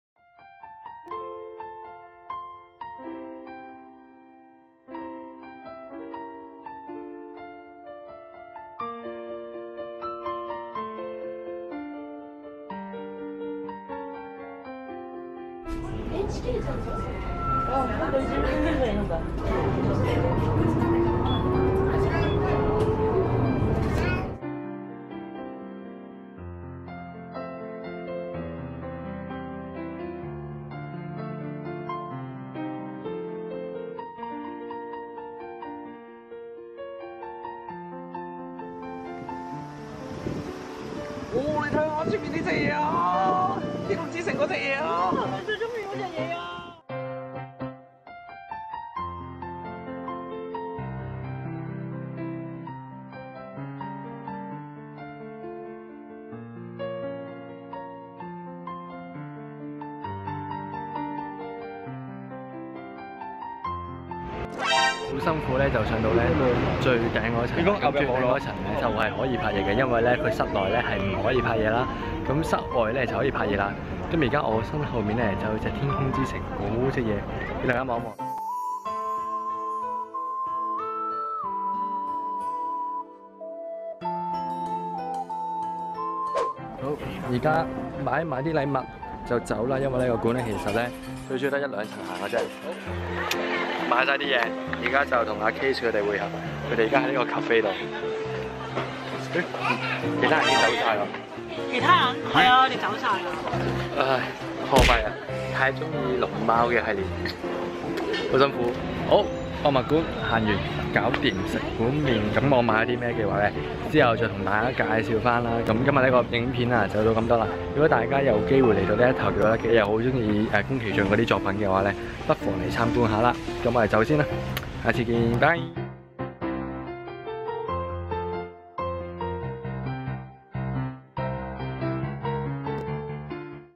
TOTORO TOTORO~~ 宮崎駿三鷹之森吉卜力美術館 ►日本東京之旅 [Hins Vlog]
今次去日本東京個邊 緊係要去睇我最鍾意既龍貓啦！！宮崎駿博物館冇理由唔去！！

有任何意見或者想有野同我講既 記住下面留言呀！